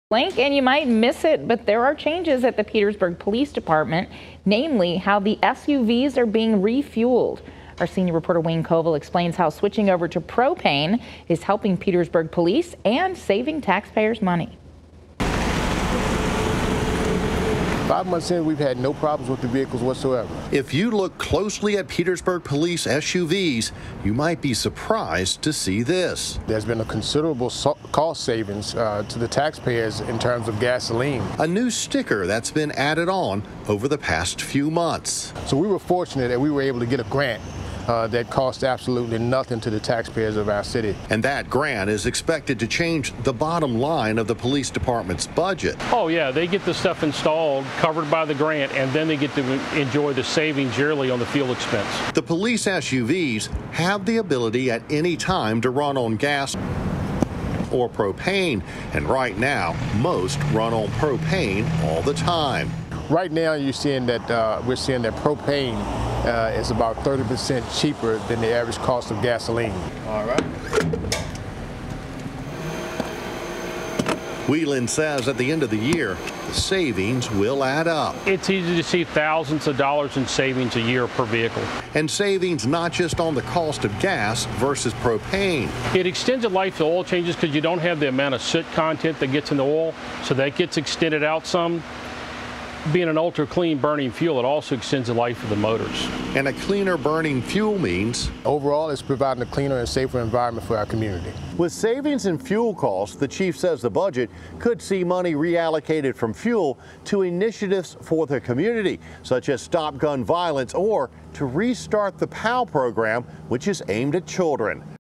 Petersburg Police pumped over money-saving move
Petersburg Police hope to save the city thousands of dollars a year in gas and car maintenance costs with its new fleet of patrol SUVs that can run on either gasoline or propane.